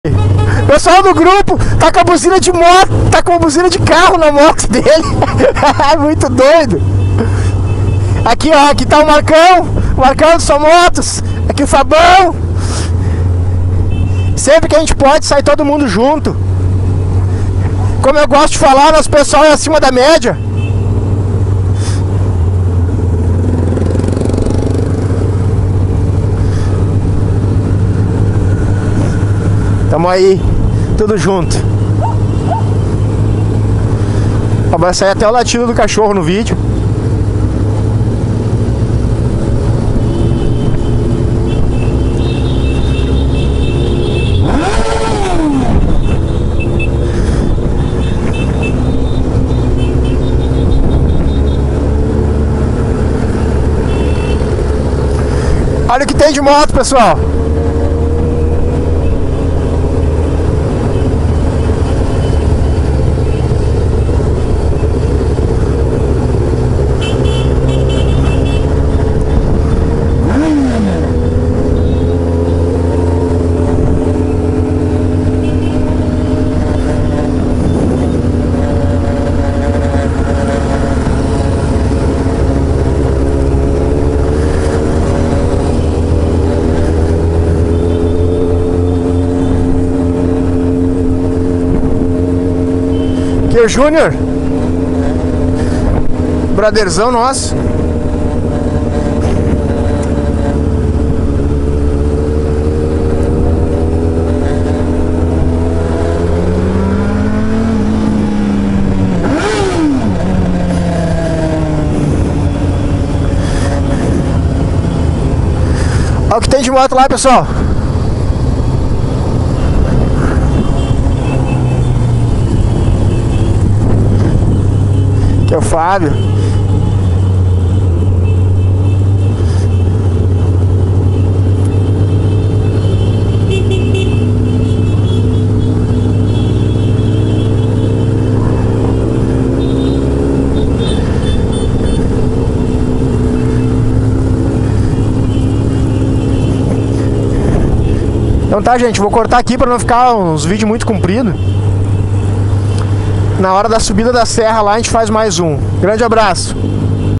Ao lado dos Irmãos
Curtindo a 10ª Moto Romaria com os amigos irmãos de estrada